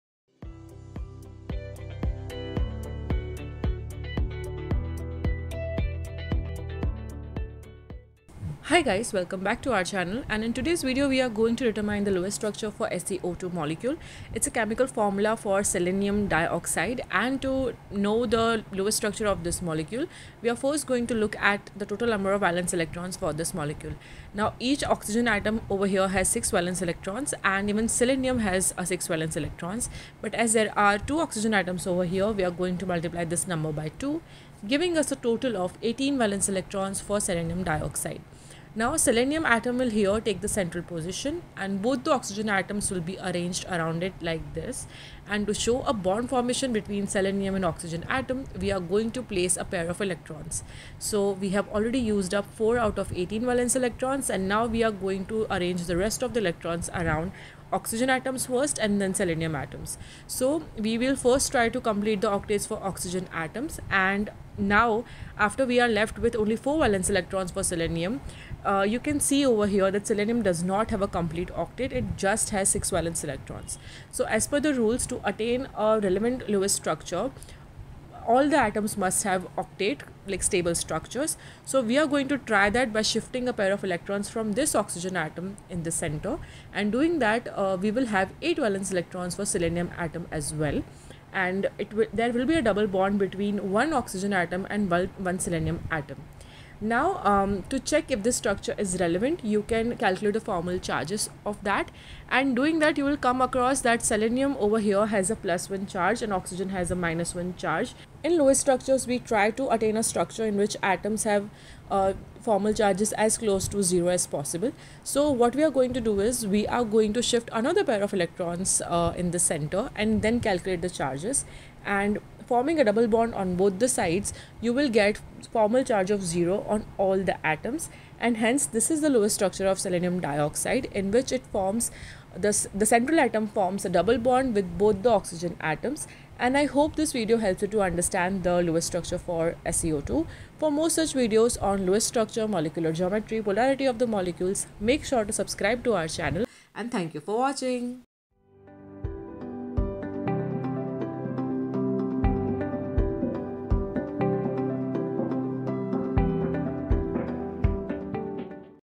SeO2 Lewis Structure (Selenium Dioxide)
Hello Guys !

Selenium Dioxide is one of those molecules that can confuse you when it comes to determining its Lewis Structure. Although it is a linear molecule, one should go through all the steps in order to get the right Lewis Structure. In this video we help you find out the relevant and accepted Lewis Structure for SeO2.

In this video we are going to look at the total number of valence electrons, bonds formed between the atoms and the formal charges on individual atoms.